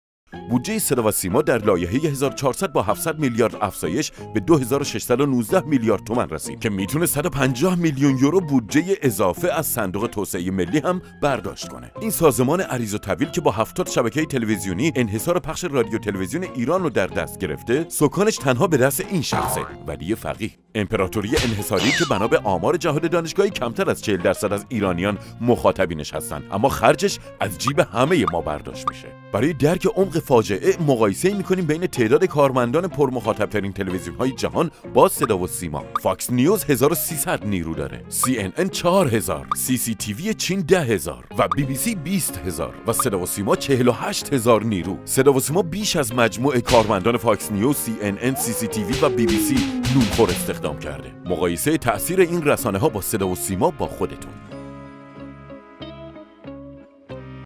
صدا و سیما غول بی‌خاصیت
می‌دونید تفاوت شهرداری با صدا و سیمای ایران در چیه؟ شهرداری آشغال جمع می‌کنه. صدا_و_سیما آشغال پخش می‌کنه. شما چند درصد از تماشای تلویزیون‌تون رو به برنامه‌های صدا و سیما اختصاص می‌دید؟